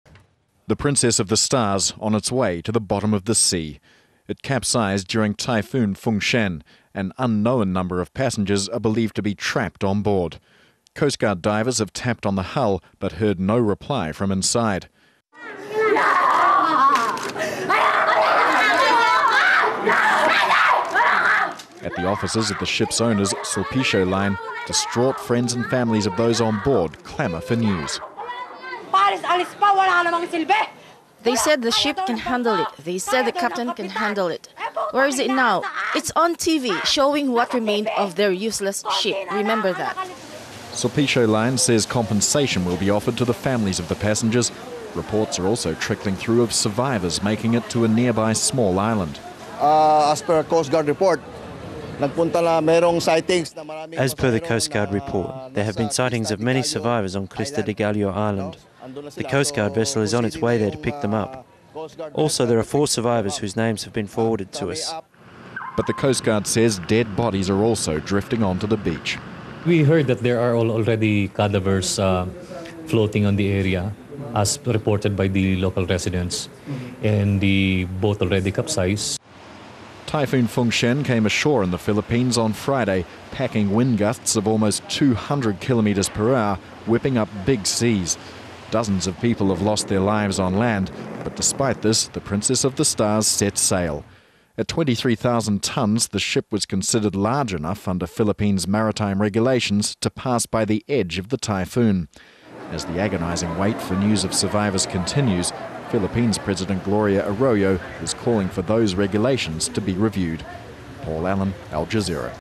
Search for Philippine ferry survivors - June 23 2008
Just a few survivors of a ferry disaster in the Philippines have made their way to shore.

The "Princess of the Stars" sank in heavy seas caused by Typhoon Fengshen.

Al Jazeera's Paul Allen reports